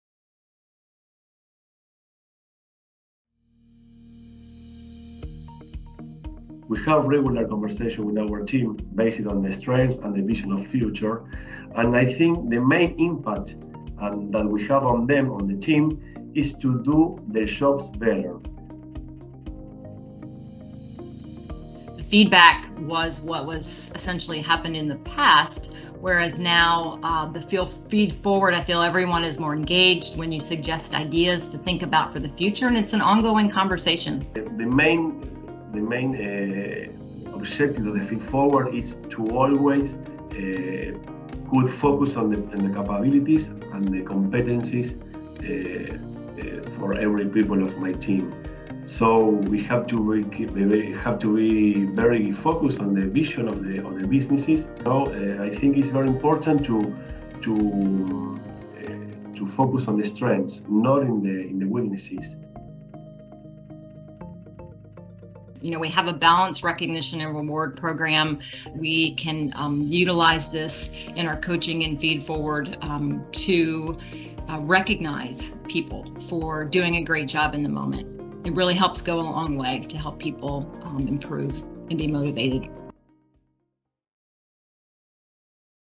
How we inspire confidence and capability in our people
Delivering life-changing medicines to patients is a responsibility we take seriously. It requires us all to live our Values and behaviors by taking accountability for our actions, the success of others and our organisation to be the best that we can possibly be. Hear from two of our employees about how we use everyday coaching and feedforward to support each other along the way.

Discover more ways that our Values and behaviors come to life in the work we do